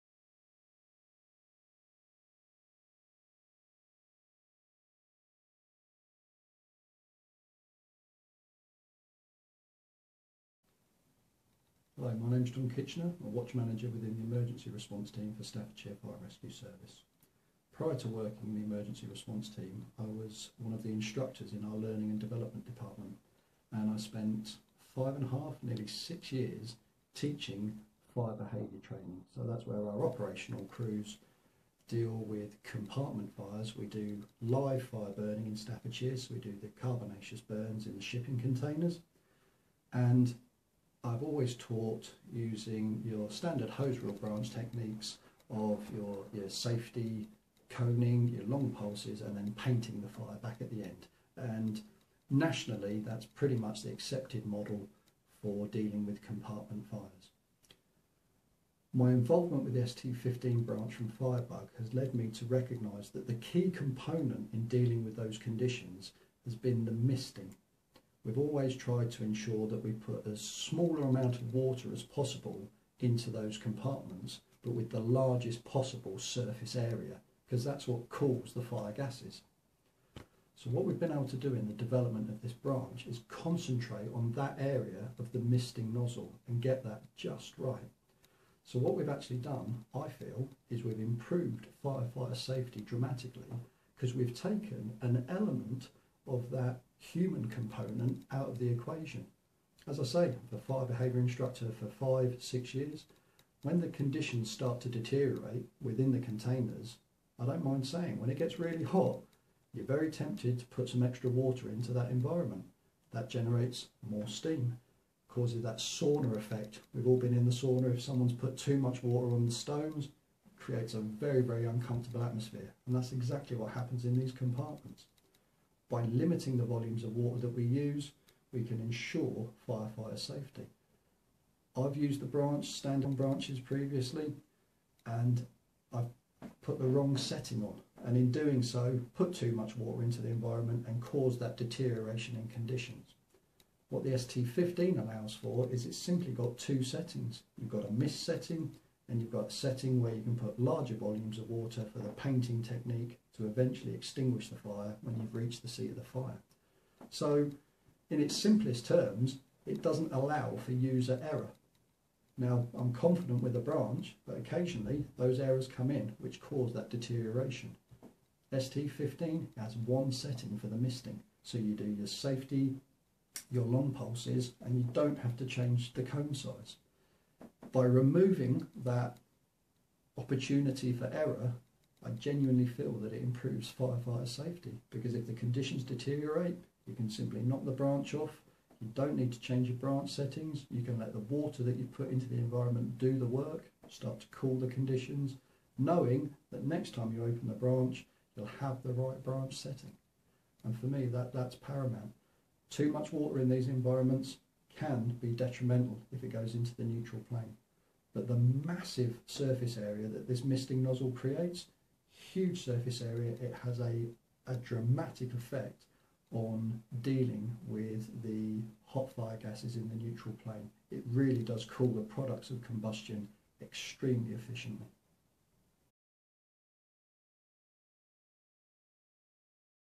"Improved fire fighter safety, dramatically" - Watch Manager
'Key component of compartment fire safety is "mist", the idea is to use as small amount of water as possible with the largest surface area' and more from John Kitchener of Staffordshire Fire & Rescue Services. As a Watch Manager with many years experience John talks us through the MistNozzle ST15 fire fighting branch by FireBug. Hear his thoughts.